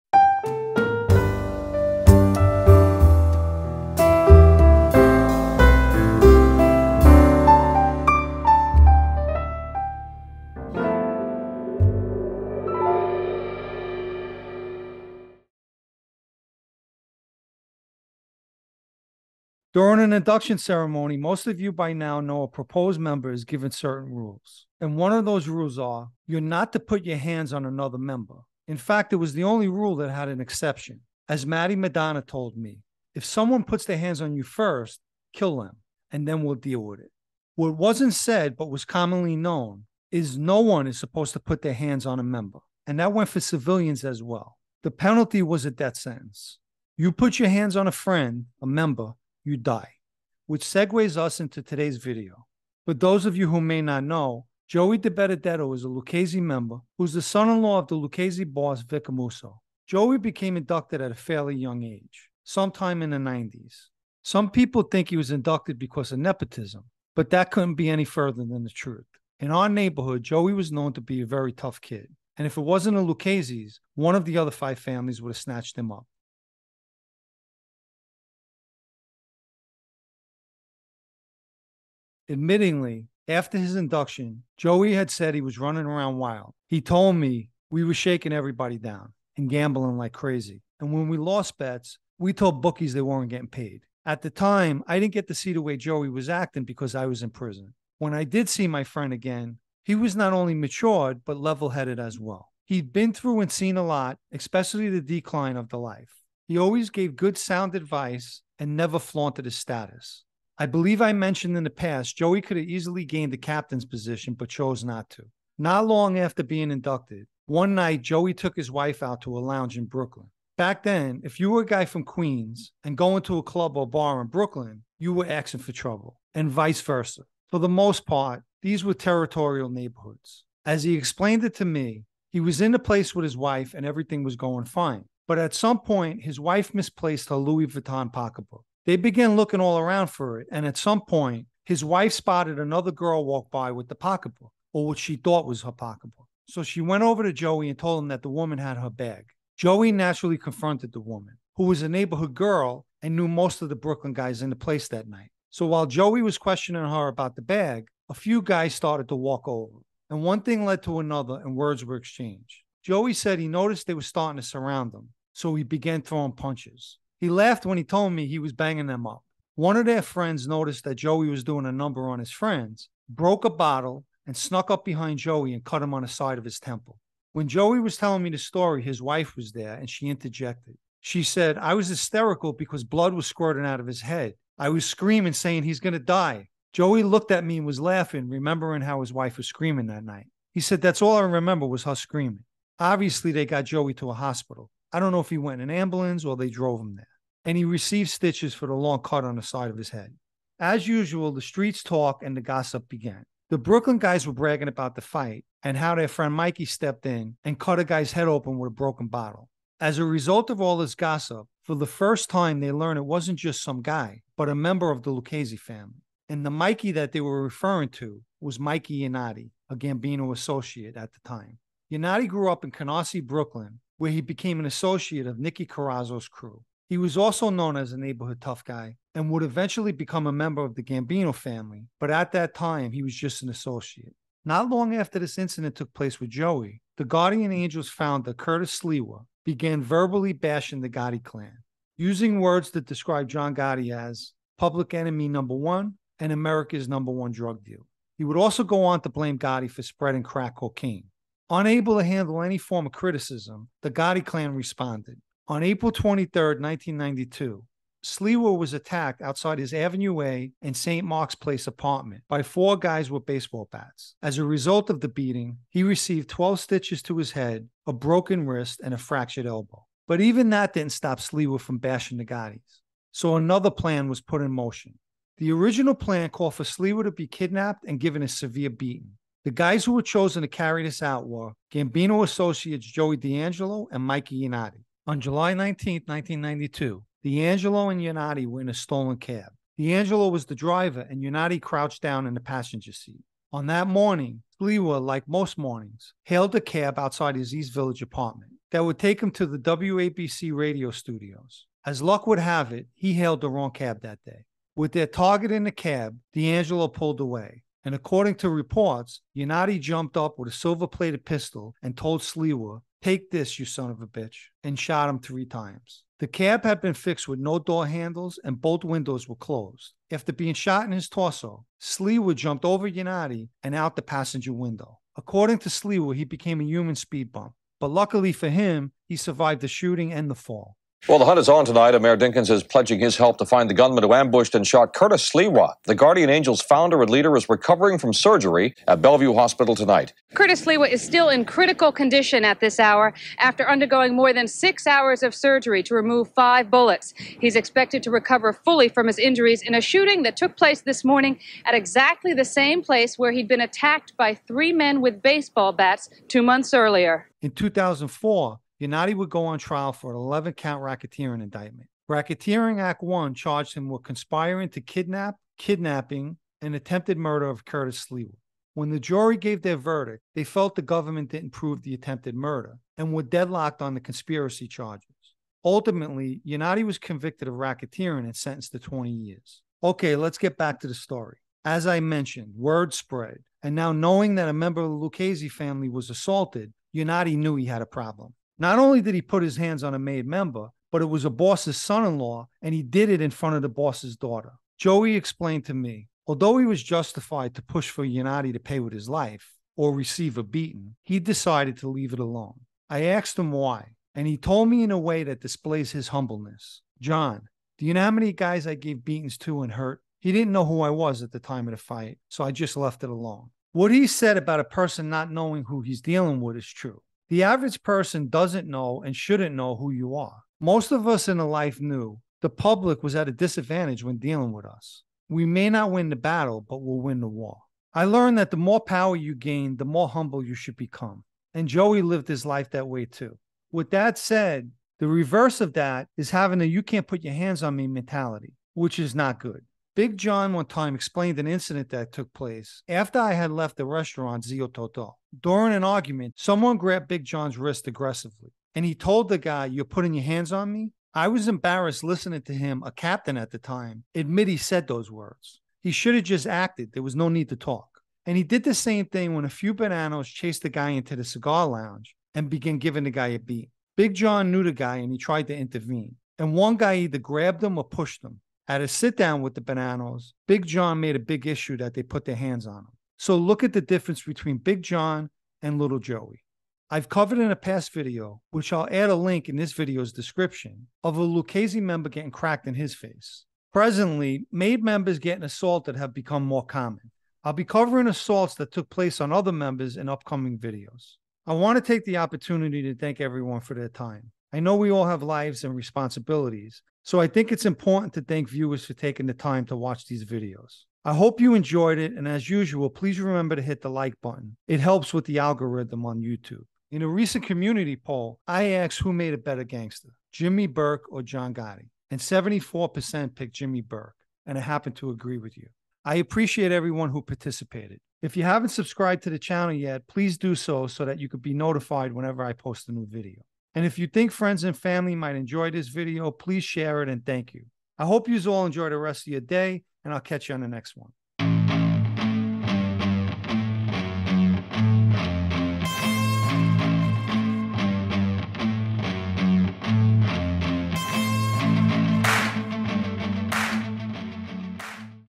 Putting hands on a [ made member ]
Detailing an incident that took place years ago when an associate put his hands on an inducted member. A violation in that life, one with serious consequences. However, Lucchese member, Joey Dibenedetto chose to display humbleness and not retaliate.

If you would like to read the Sitdownnews blog, you can do so